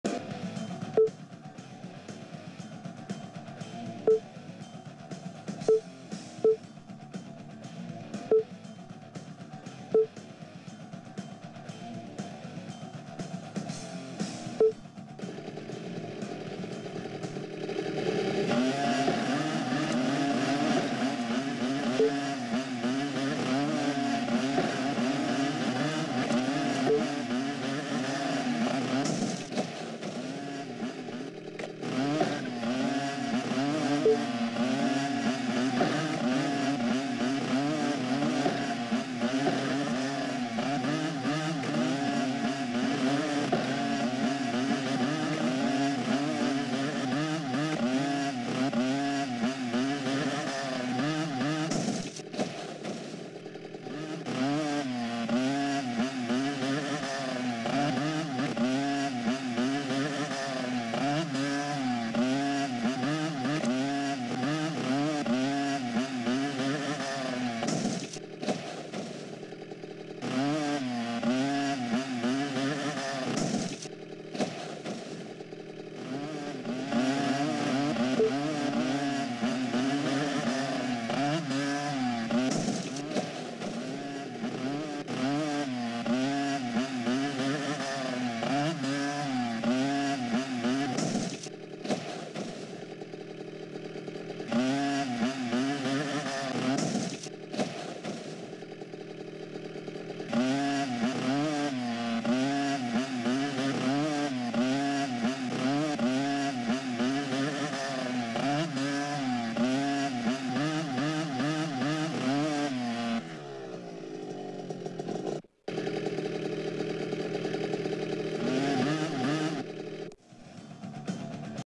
Motosiklet Yarışı Oyna - Akrep Oyun
Oyuna başlarken öncelikle cinsiyetimizi seçiyoruz, daha sonra kıyafetimizin rengini belirliyoruz, son olarak da motosikletimizin rengini seçiyoruz. Zorlu pistte rakiplerimizi geride bırakıp ilk önce bitiş çizgisine ulaşmalıyız. Motosikletimizi yön tuşlarıyla ya da W,A,S,D tuşlarıyla yönlendirirken, boşluk tuşuyla zıplayabilir, Z,X ve C tuşlarına basarak motorumuzla artistik hareketler yapabiliriz.